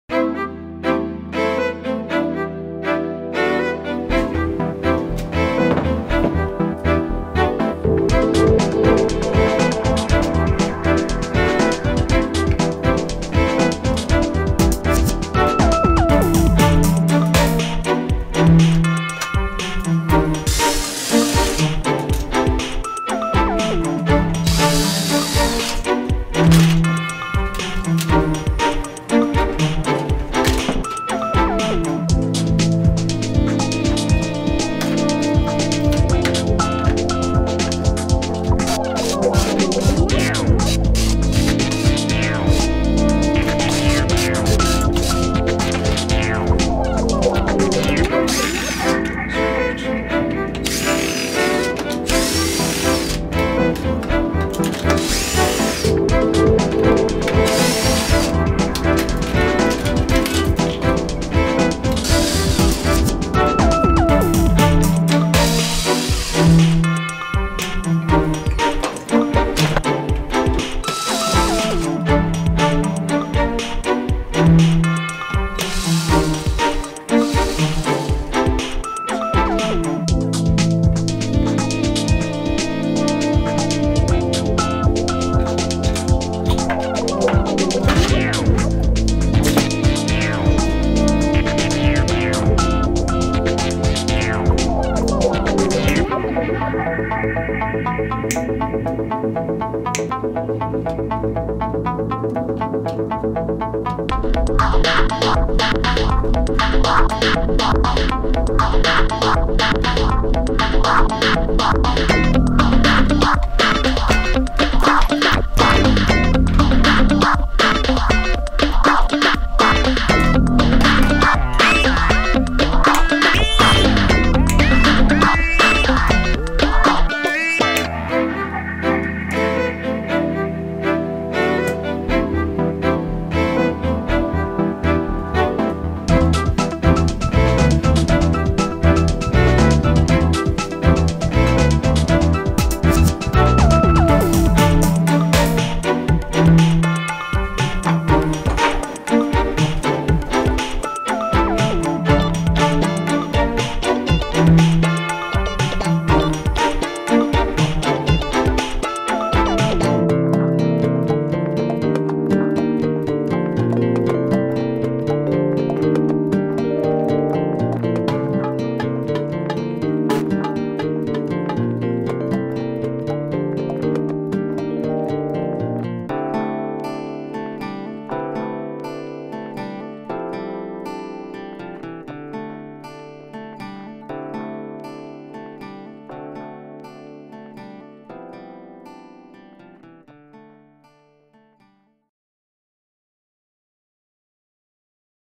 How to make a lamp out of an Apple G4 iMac. Part 1 Disassembly
Order the materials and video instructions necessary to turn your own G4 iMac into an iLamp